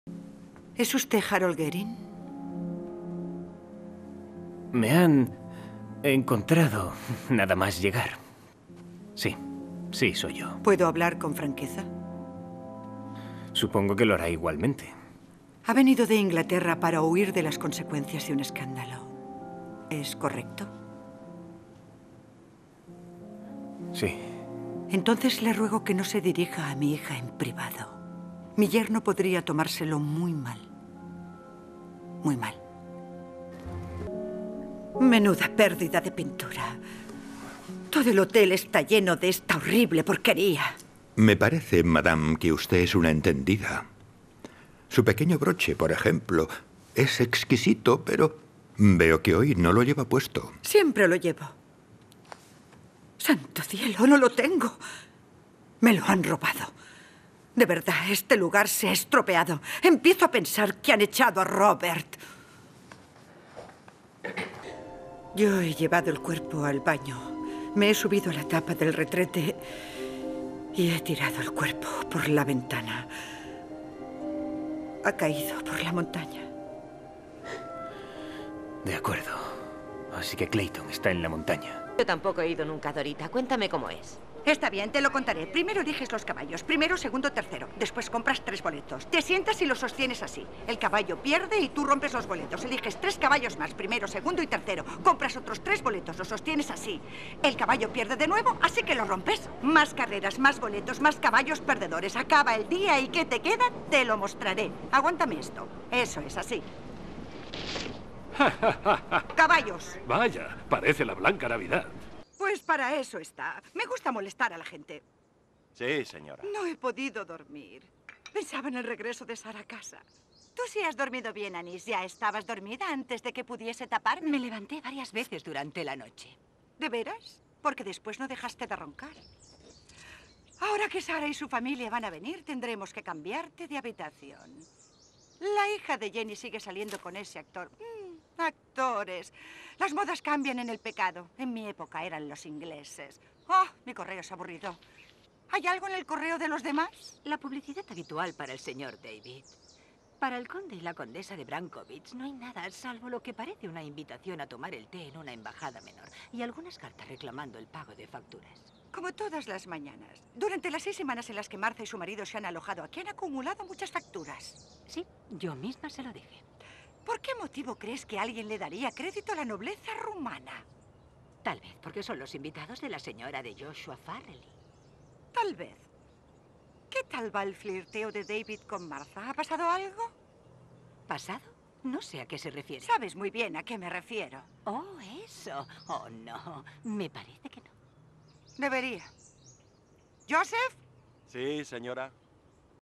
ESPERANZA GRACIA - ACTRIZ DE DOBLAJE
Esperanza Gracia, profesora en Set d'Acció de los cursos de Doblaje, es una de las actrices de doblaje de mayor reputación en nuestro país. En este vídeo nos deja una muestra de algunos de los personajes a los que ha puesto voz.